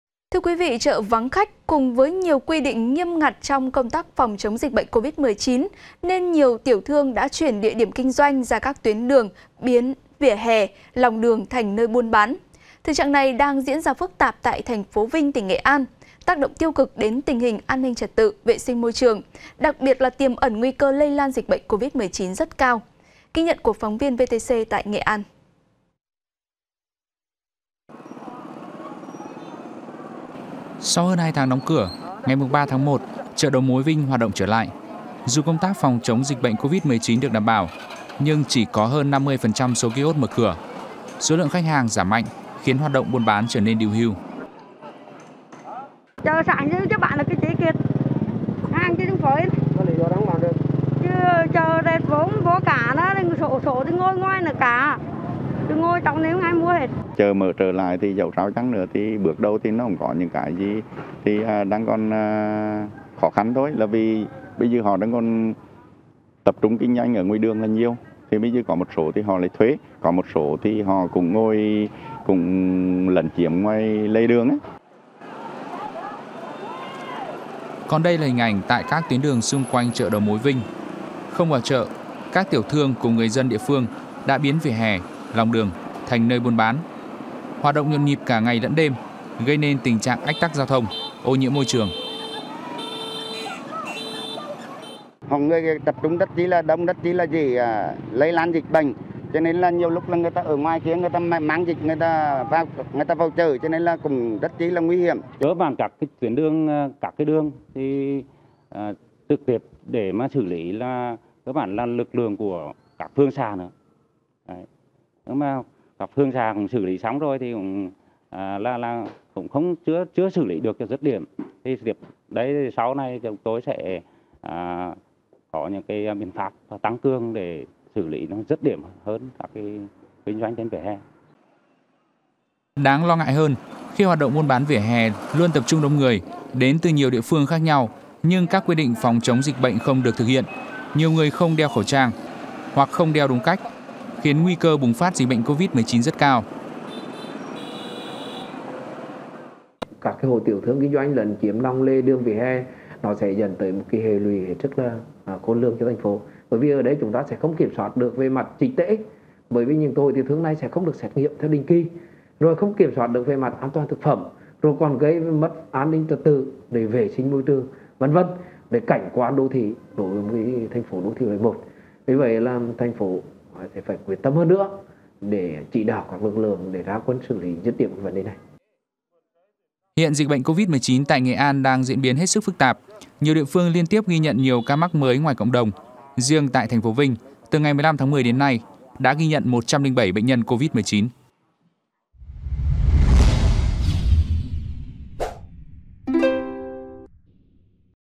Nghệ An: Nguy cơ lây nhiễm dịch bệnh Covid-19 từ buôn bán vỉa hè | VTC14
VTC14 | NGHỆ AN: NGUY CƠ LÂY NHIỄM DỊCH BỆNH COVID-19 TỪ BUÔN BÁN VỈA HÈ
Chợ vắng khách, cùng với nhiều quu định nghiêm ngặt trong công tác phòng chống dịch bệnh covid-19 nên nhiều tiểu thương đã chuyển địa điểm kinh doanh ra các tuyến đường, biến vỉa hè, lòng đường thành nơi buôn bán. Thực trạng này đang diễn ra phức tạp tại TP Vinh (Nghệ An) tác động tiêu cực đến tình hình an ninh trật tự, vệ sinh môi trường, đặc biệt là tiềm ẩn nguy cơ lây lan dịch bệnh covid-19 rất cao.